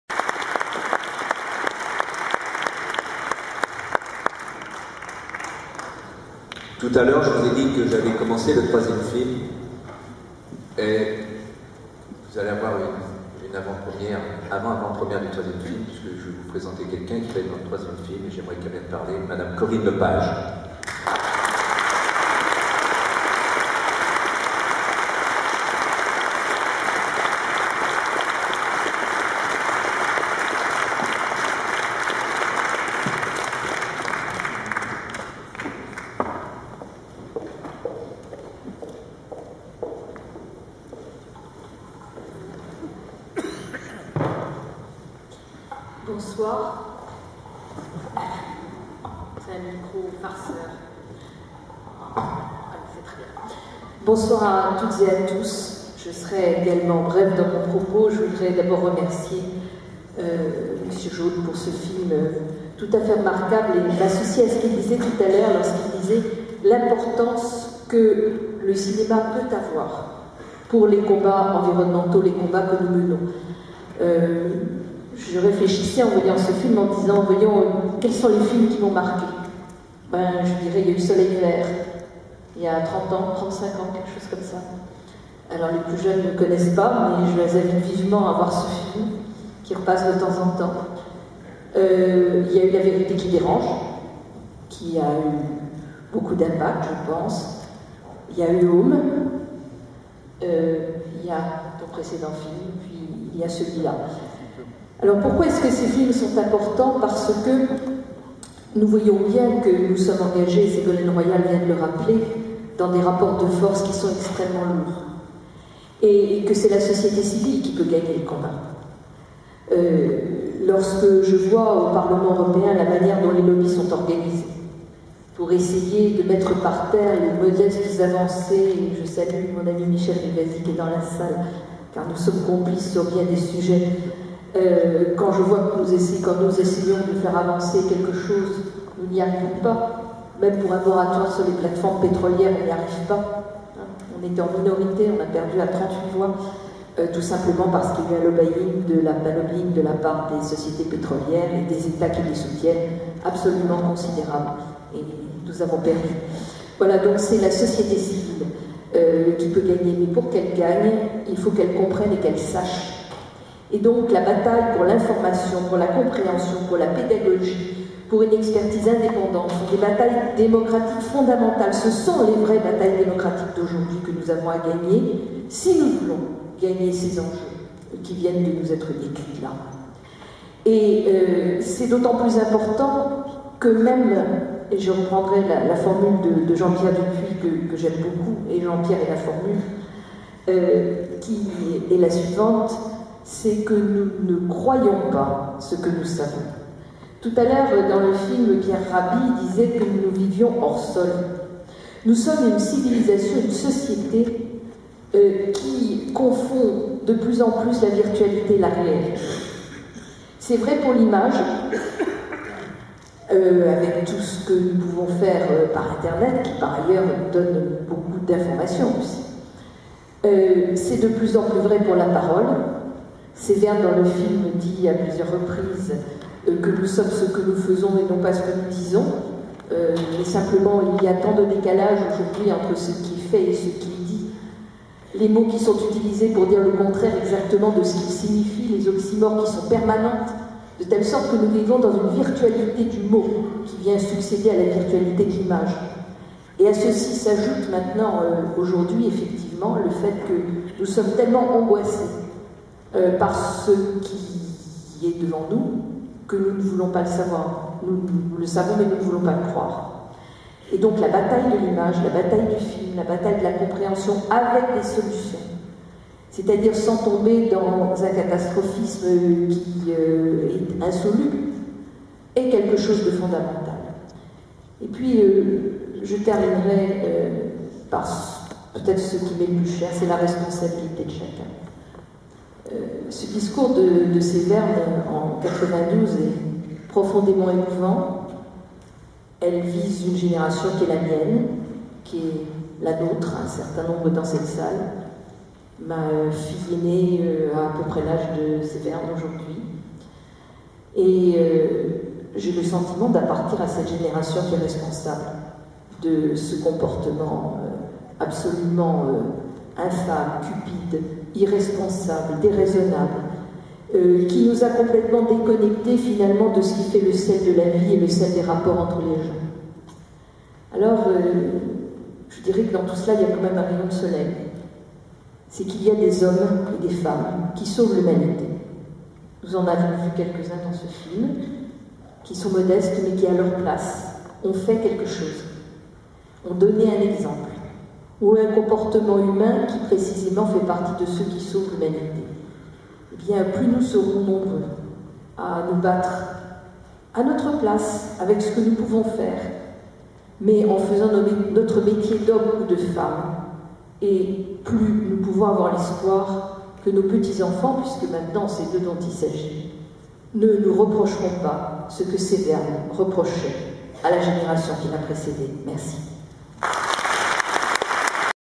Avant première du film Severn - 4) Corinne Lepage
Vidéo enregistrée lors de l'avant première du film de Jean-Paul Jaud "Severn, la voix de nos enfants" à l'Unesco le 8 novembre 2010 : intervention de Corinne Lepage.

Plus d'informations sur cet évènement sur le site du Retour à la Terre